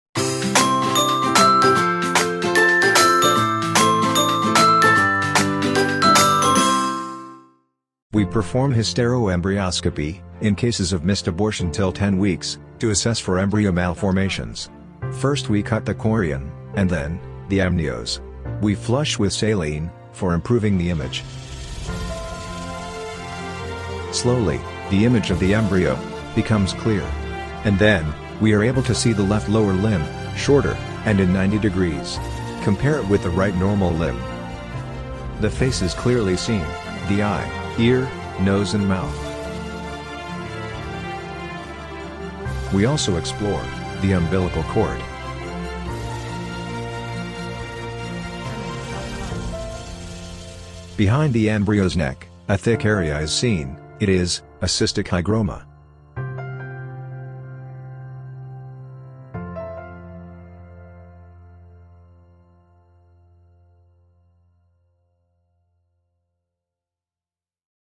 9w embryoscopy limb malformation
During the last 3 years in our center we have been offering to the patients with a missed abortion (till 10w) to perform a hystero-embryoscopy for assessing if there are any malformations and also to check for the implantation wall of the pregnancy. We also offer them to take a non contaminated biopsy of the embryo for a genetic study.
You only need diagnostic hysteroscopy equipment.
In this case, a 9w embryo with left lower limb malformation a cystic hygroma
When we finish the embryoscopy we perform the aspiration focussed only on the implantation wall without touching the other uterine walls. By this, we do not damage the uterine walls avoiding unnecessary complications such as adhesions.